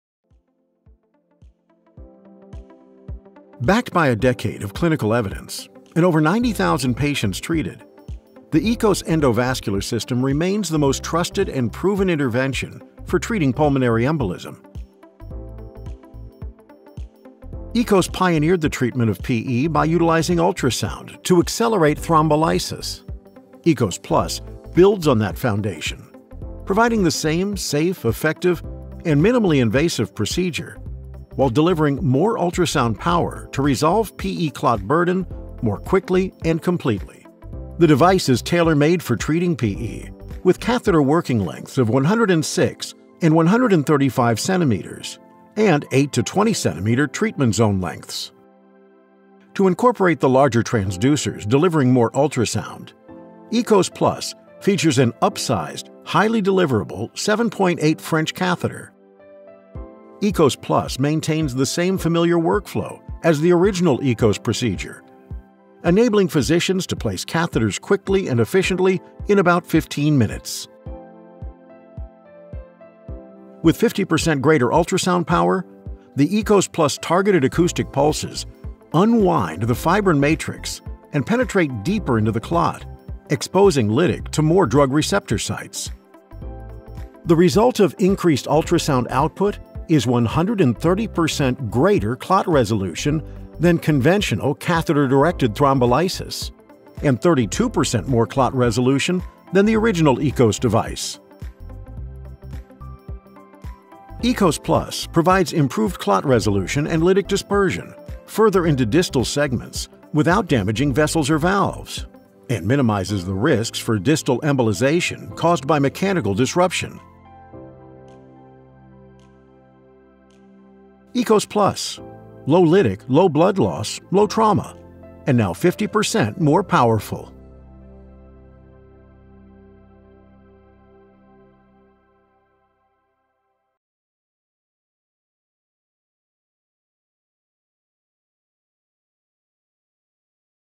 EKOS+ Endovascular System: How it Works
Learn how EKOS+ builds upon the legacy of EKOS with more ultrasound power and greater clot dissolution.

Boston Scientific is dedicated to transforming lives through innovative medical solutions that improve the health of patients around the world.

PI-1374208-AA